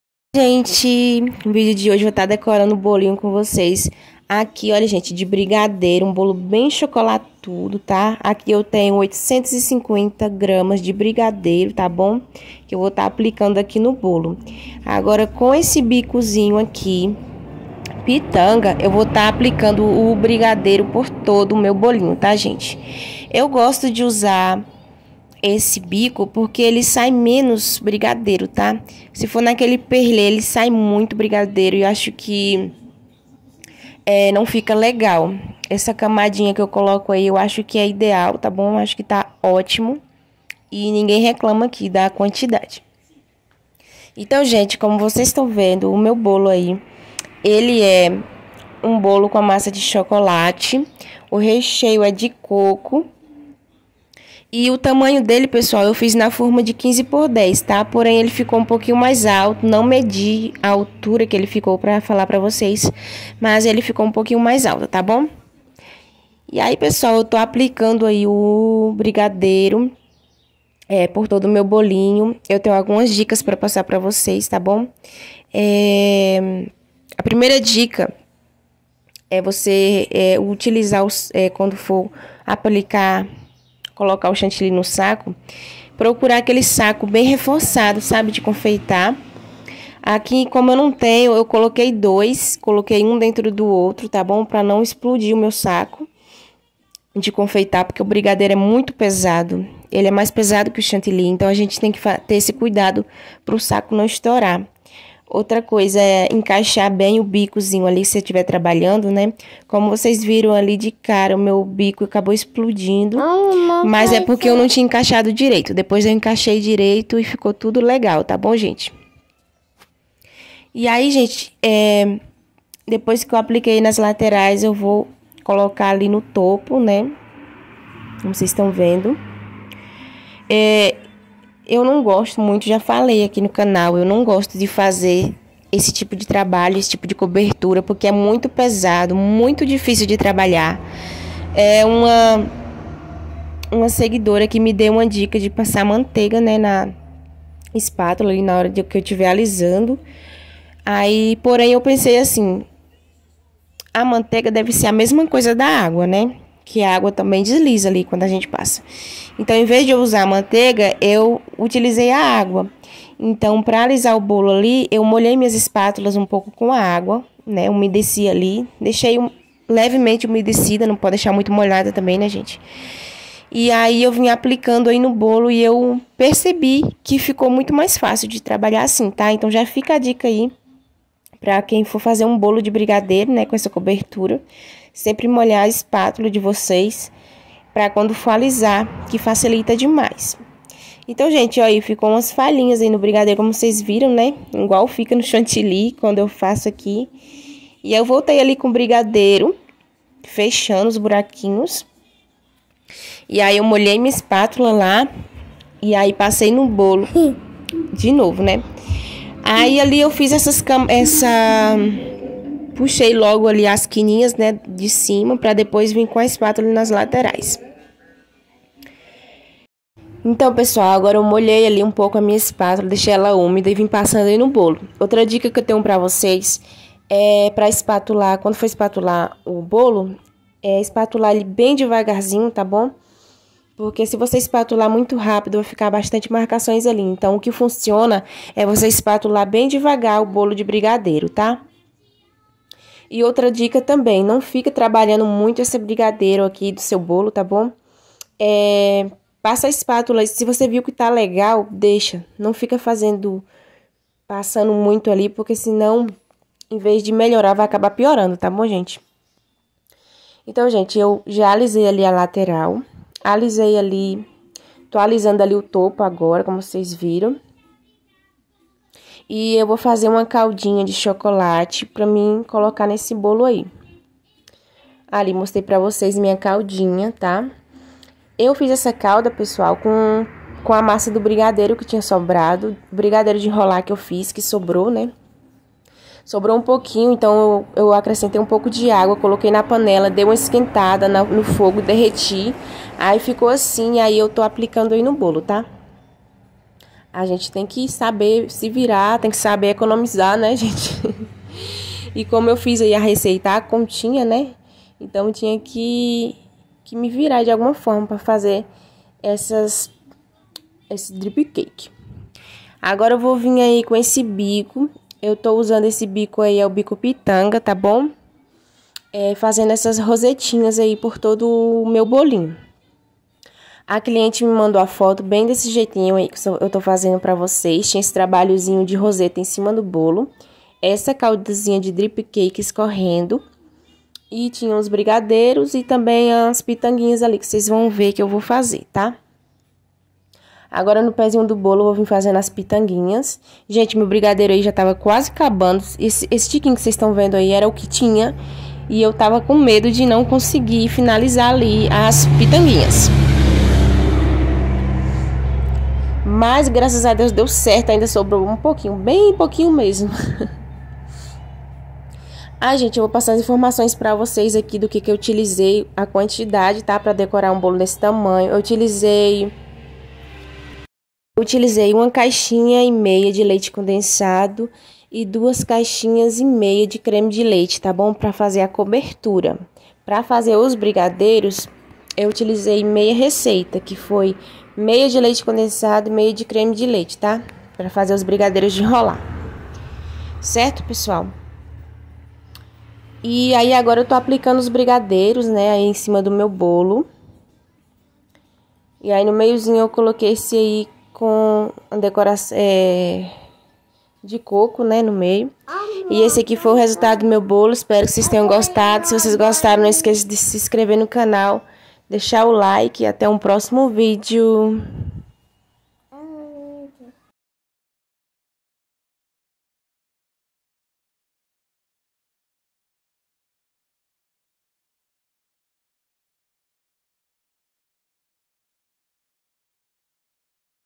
COMO DECORAR BOLO COM COBERTURA DE CHOCOLATE/ BOLO CHOCOLATUDO
oi gente no vídeo de hoje trouxe esse bolo bem chocolatudo pra vocês, onde eu dou vários dicas importantes pra quem for fazer um bolo nesse estilo. Assista até o final o vídeo pra você não errar quando for decorar um bolo assim!!!

*eu utilizei pra cobrir todo o bolo e fazer as rosetas
850g de brigadeiro

ingredientes da cobertura:
1 caixinha e meia de leite condensado
2 caixinha e meia de creme de leite
60g de chocolate 50%
1 colher de margarina

*quantidade de ingredientes pra fazer o brigadeiro de enrolar:
1/2 caixa de leite condensado
1/2 caixinha de creme de leite
25g de chocolate 50%
1/2 colher de margarina

✓me rendeu 15 brigadeiros

Beijos meus amores 😗 espero ter ajudado 💗